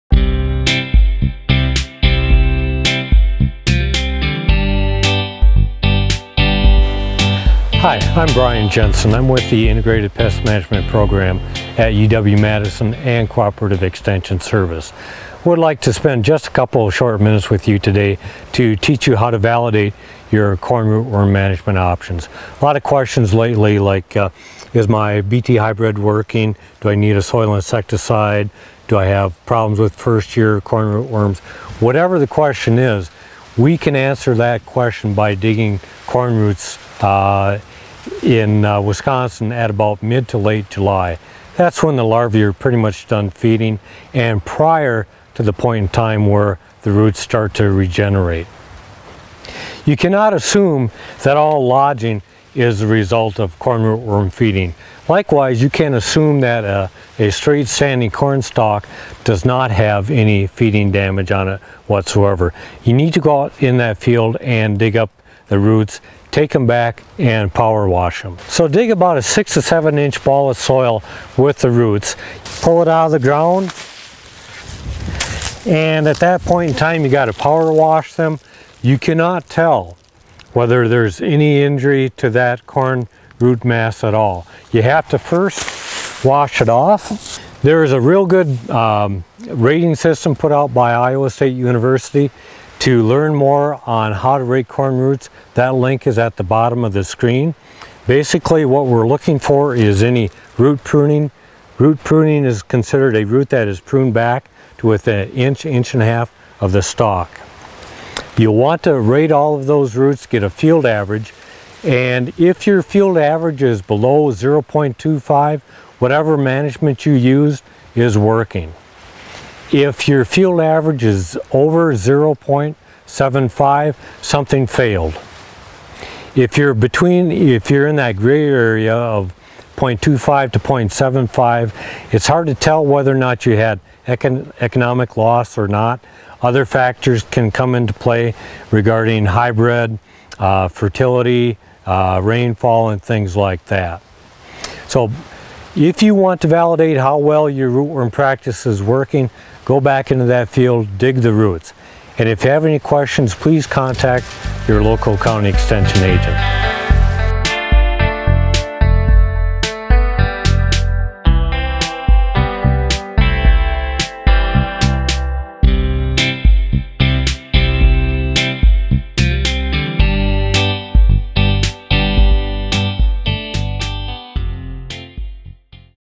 Corn rootworm: How to validate your management decision
Mid to late July in Wisconsin is the time you can validate your corn rootworm management decisions by checking for root damage. This is the time period just after feeding damage would have occurred, and just before regrowth would start to mask the damage.

Bryan Jensen with the University of Wisconsin Integrated Pest Management Program takes you into a corn field to show you how to dig corn roots to use for rating damage.

Also view this root rating video.